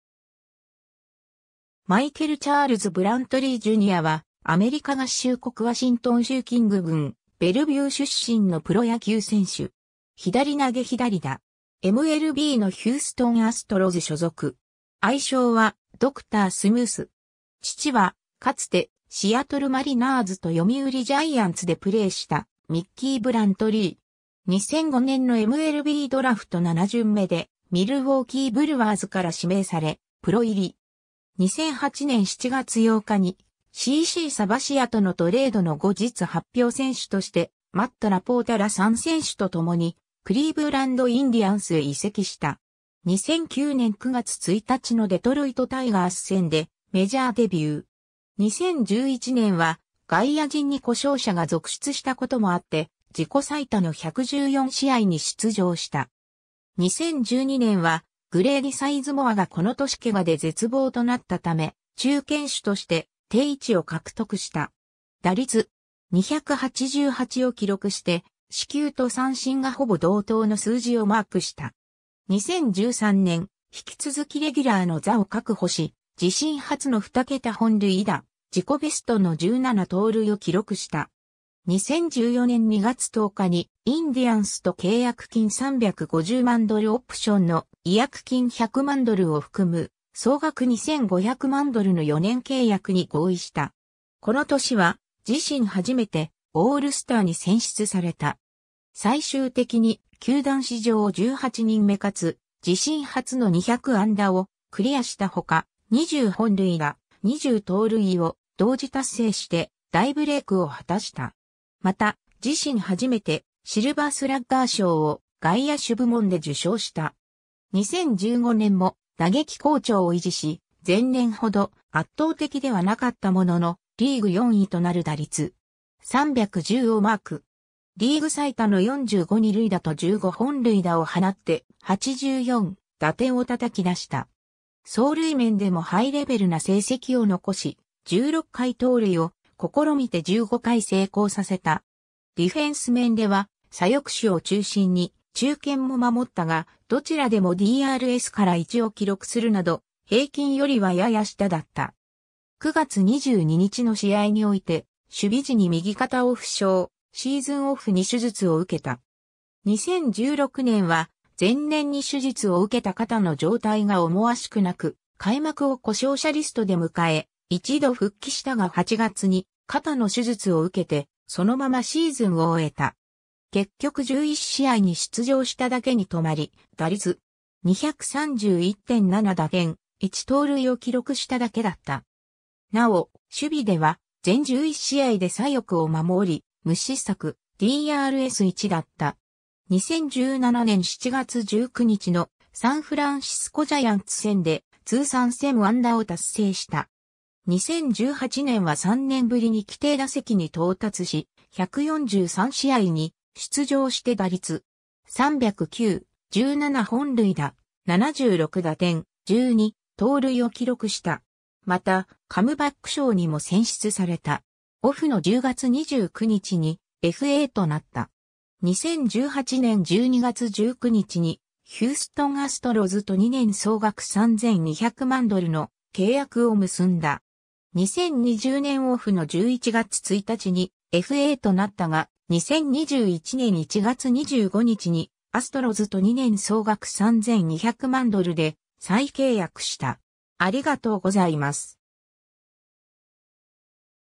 マイケル・ブラントリー
マイケル・チャールズ・ブラントリー・ジュニア（Michael Charles Brantley, Jr., 1987年5月15日 - ）は、アメリカ合衆国ワシントン州キング郡ベルビュー出身のプロ野球選手（外野手）。
左投左打。
MLBのヒューストン・アストロズ所属。
 愛称はドクタースムース（Dr.Smooth）。
 父は、かつてシアトル・マリナーズと読売ジャイアンツでプレーしたミッキー・ブラントリー。
 2005年のMLBドラフト7巡目（全体205位）でミルウォーキー・ブルワーズから指名され、プロ入り。
 2008年7月8日にCC・サバシアとのトレードの後日発表選手として、マット・ラポータら3選手と共にクリーブランド・インディアンスへ移籍した。
 2009年9月1日のデトロイト・タイガース戦でメジャーデビュー。
 2011年は外野陣に故障者が続出したこともあって、自己最多の114試合に出場した。
 2012年は、グレイディ・サイズモアがこの年怪我で絶望となったため中堅手として定位置を獲得した。
打率.288を記録して、四球と三振がほぼ同等の数字をマークした。
 2013年、引き続きレギュラーの座を確保し、自身初の二桁本塁打・自己ベスト（当時）の17盗塁を記録した。
 2014年2月10日にインディアンスと契約金350万ドル+オプションの違約金100万ドルを含む、総額2500万ドルの4年契約（2018年・1100万ドルのオプション付き）に合意した。
この年は、自身初めてオールスターに選出された。
最終的に、球団史上18人目かつ自身初の200安打をクリアしたほか、「20本塁打・20盗塁」を同時達成して大ブレイクを果たした。
また、自身初めてシルバースラッガー賞を外野手部門で受賞した。
 2015年も打撃好調を維持し、前年ほど圧倒的ではなかったものの、リーグ4位となる打率.310をマーク。
リーグ最多の45二塁打と15本塁打を放って、84打点を叩き出した。
走塁面でもハイレベルな成績を残し、16回盗塁を試みて15回成功させた (成功率94％) 。
ディフェンス面では、左翼手を中心に中堅も守ったが、どちらでもDRS - 1を記録するなど、平均よりはやや下だった。
9月22日の試合において守備時に右肩を負傷、シーズンオフに手術を受けた。
 2016年は、前年に手術を受けた肩の状態が思わしくなく、開幕を故障者リストで迎え、1度復帰したが8月に肩の手術を受けて、そのままシーズンを終えた。
結局11試合に出場しただけに留まり、打率.231・7打点・1盗塁を記録しただけだった。
なお、守備では全11試合で左翼を守り、無失策・DRS + 1だった。
 2017年7月19日のサンフランシスコ・ジャイアンツ戦で通算1000安打を達成した。
 2018年は3年ぶりに規定打席に到達し、143試合に出場して打率.309、17本塁打、76打点、12盗塁を記録した。
また、カムバック賞にも選出された。
 2018年12月19日にヒューストン・アストロズと2年総額3200万ドルの契約を結んだ。
 2020年オフの11月1日にFAとなったが、2021年1月25日にアストロズと2年総額3200万ドルで再契約した。